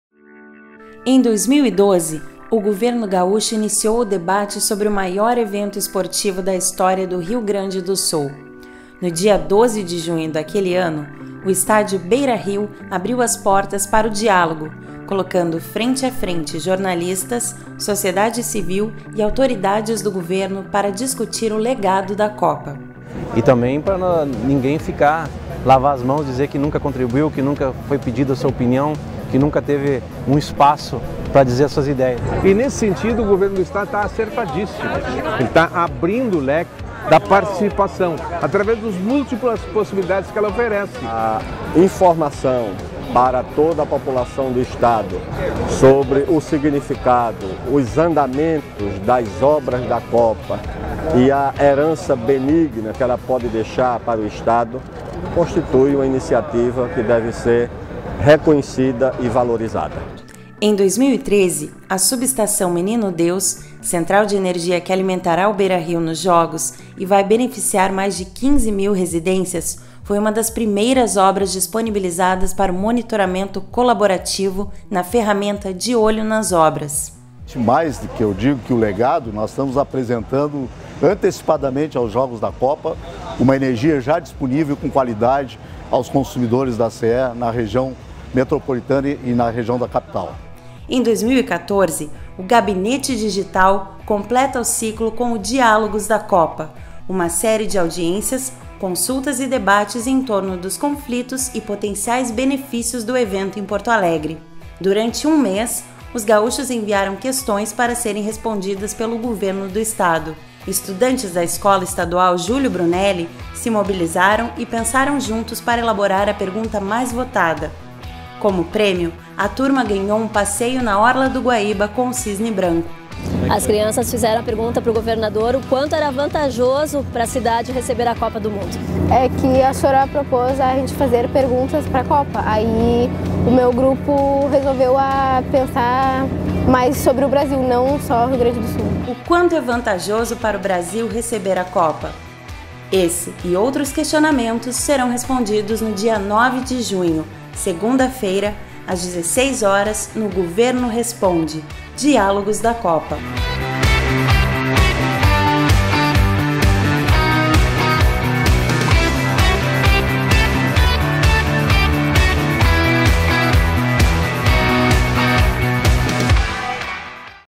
Governo Responde: Diálogos da Copa debate sobre o Mundial no Brasil
Não é de hoje que o Gabinete Digital debate com a sociedade o tema da Copa do Mundo no Brasil. Desde 2012 o Governo do Estado do RS tem procurado dialogar entre vários eixos da sociedade para construirmos um evento mais democrático. O Governo Responde: Diálogos da Copa no dia 9 de junho (segunda-feira), às 16h, com a presença do governador Tarso Genro e diversas representações da sociedade civil fará um último debate sobre o tema.